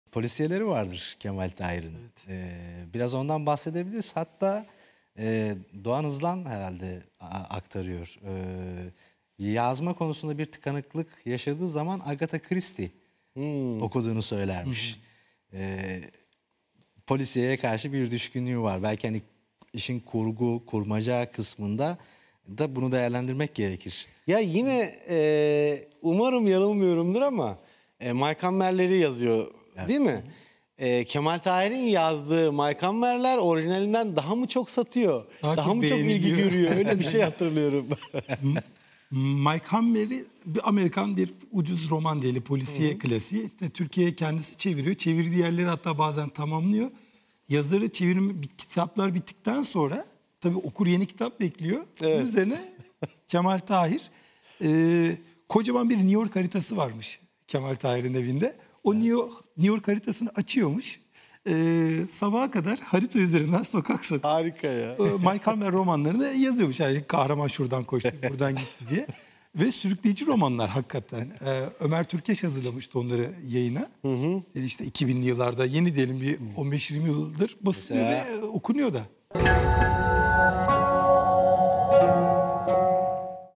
Kemal Tahir ve Polisiye Roman | Kelimeler ve Şeyler
Kemal Tahir'in çevirisini yaptıktan sonra, F. M. İkinci takma adıyla kaleme aldığı ve aslından daha iyi olmasıyla anılan "Mayk Hammer" romanlarının yazım sürecini dinliyoruz. ✍️